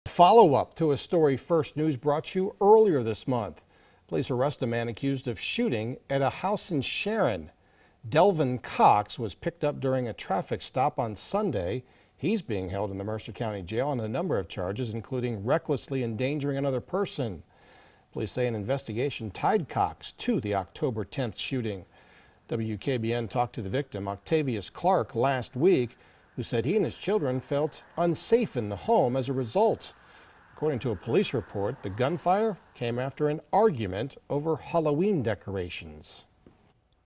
Suspect arrested, accused of shooting at house in Sharon
Delvon Cox is charged with gun and reckless endangering charges